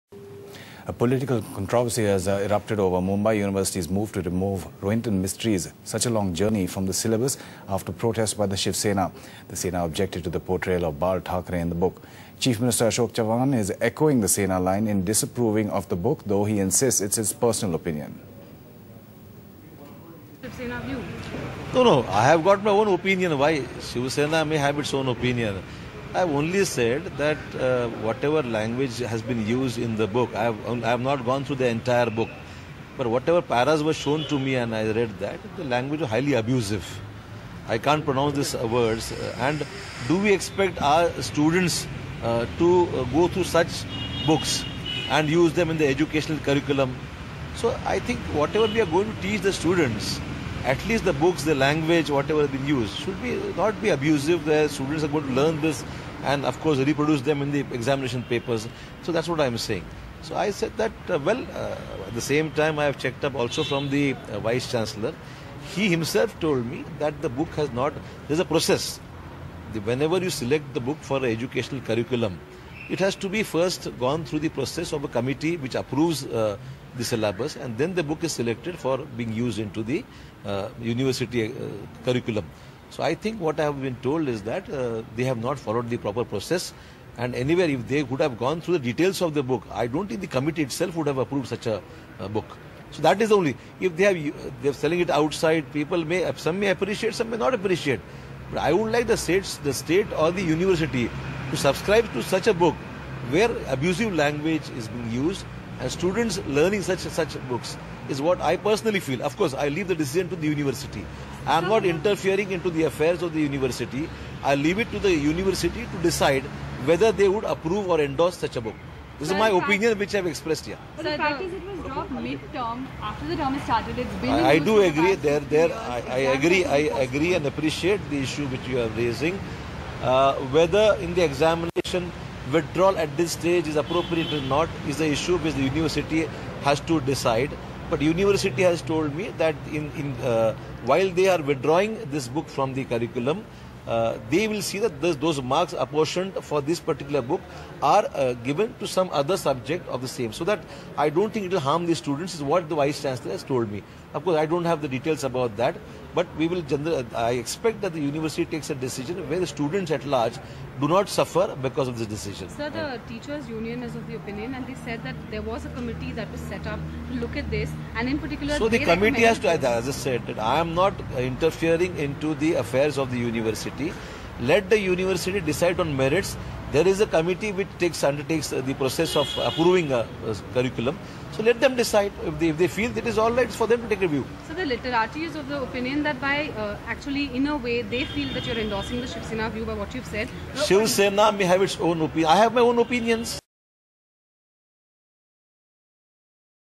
Objectionable language in Mistry's book: Chavan
Echoing Shiv Sena line on Rohinton Mistry's book "Such a long Journey" Maharashtra CM Ashok Chavan has said that he believes the book's language is objectionable.